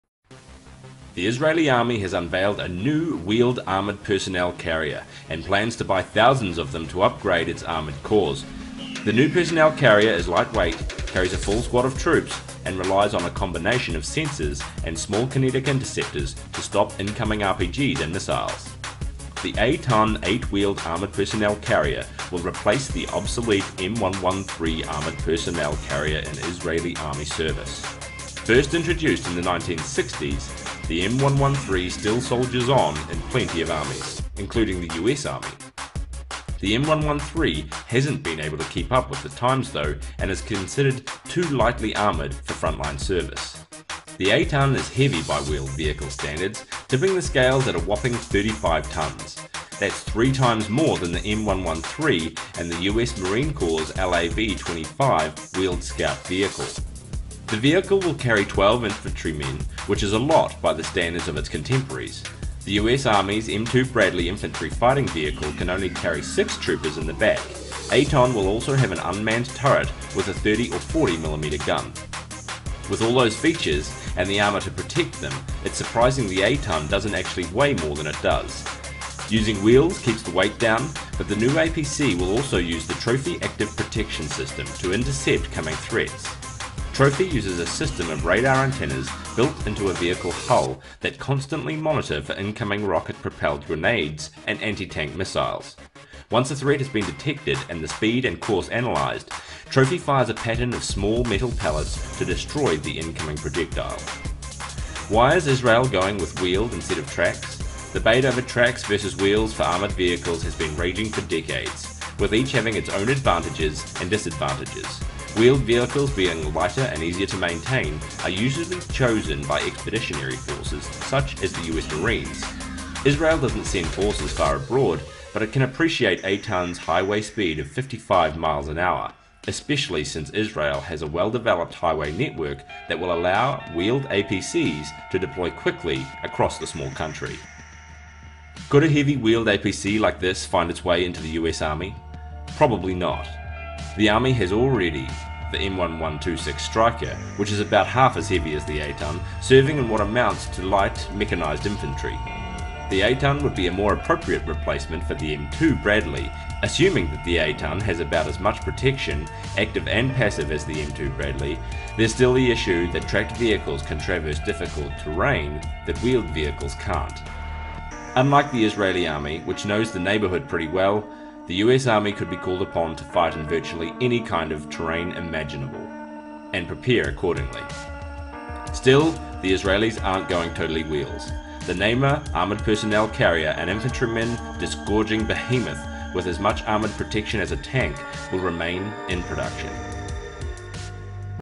'Eitan,' IDF's newest armored personnel carrier
The "Eitan" armored personnel carrier is the most advanced APC of its kind in the world, and is being developed by the Administration for Merkava and Armored Combat Vehicles in Israel's Ministry of Defense.